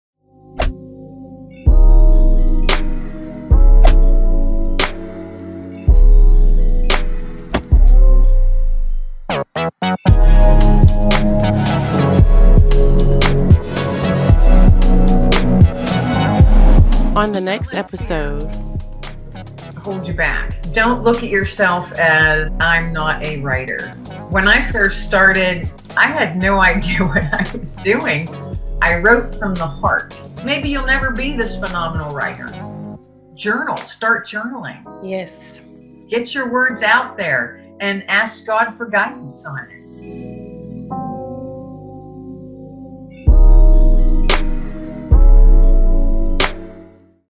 THE WOMAN I AM TODAY Kimberly Lengling Commercial
Kimberly Lengling Author, Freelance Writer, Speaker and podcast host of Let Fear Bounce. Painting pictures through the written word. Spreading nuggets of kindness and compassion into the world. Let's change the world, one story at a time! Join us as she sares about her new collaborative book venture
"When Grace Found Me Series" kimlenglingauthor.com
Thursday Feb.4th @5pm on Preach The Word Worldwide Network
and Live Stream On Nu Vizion Televizion